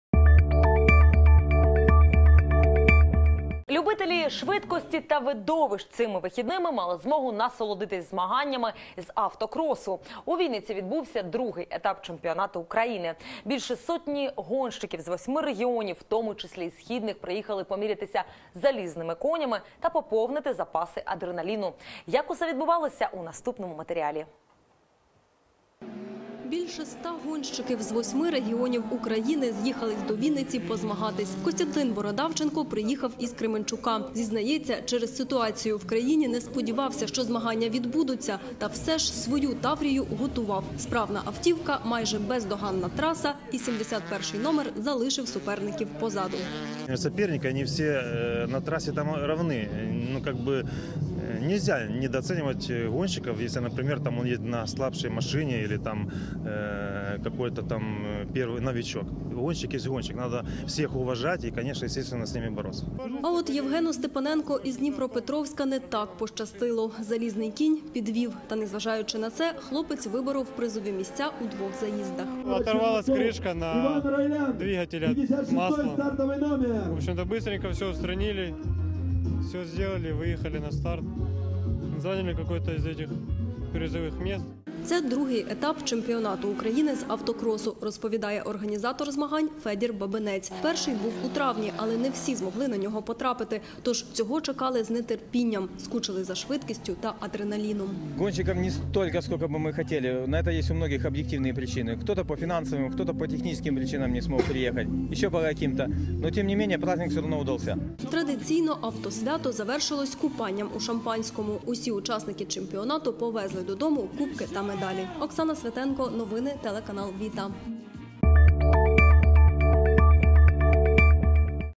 Телеканал ВІТА новини 2014-08-18 Швидкість та екстрим єднає
Любителі швидкості та видовищ цими вихідним мали змогу насолодитись змаганнями з автокросу. У Вінниці відбувся другий етап чемпіонату України. Більше сотні гонщиків з 8-ми регіонів, в тому числі і східних приїхали помірятись залізними конями та поповнити запаси адреналіну. Як усе відбувалось – у наступному матеріалі.